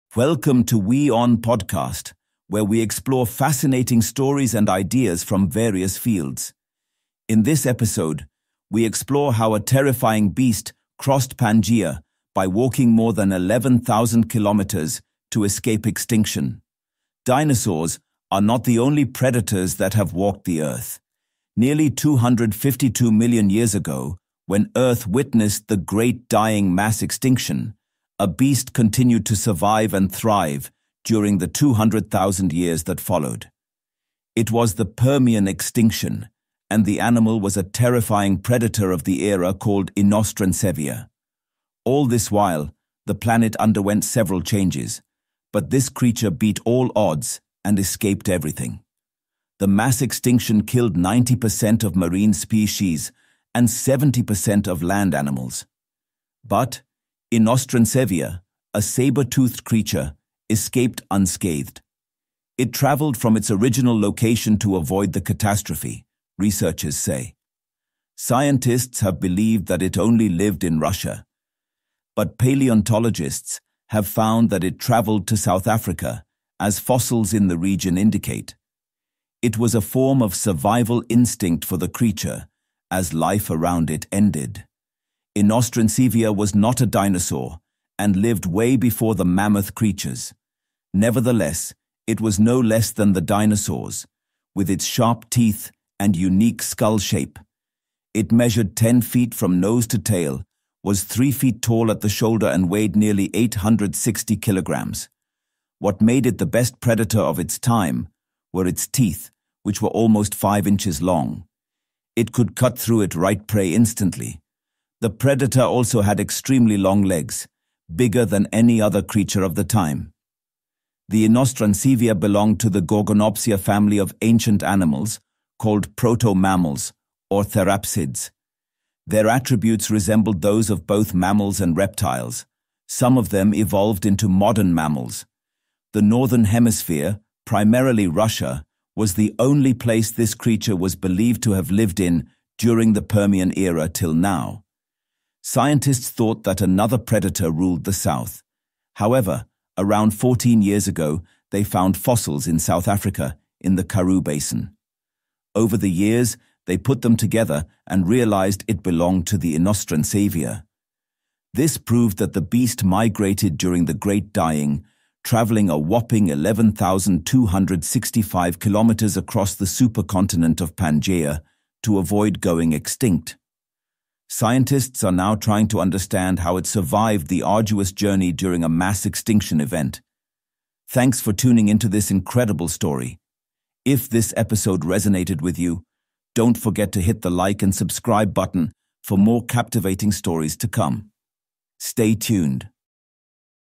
Ancient Monster’s Epic Journey Across Pangea During Earth’s Deadliest Extinction | WION Podcast
Scientists have uncovered fossils of a terrifying creature that lived long before the dinosaurs and survived Earth’s deadliest extinction event—the Great Dying. This ancient predator is believed to have walked over 11,000 kilometres across the supercontinent Pangea to escape extinction. The discovery sheds light on how early species adapted to extreme environmental changes. With powerful limbs and a fearsome appearance, this beast’s survival story is as fascinating as it is haunting. In this episode, we explore how it endured the planet’s most catastrophic era.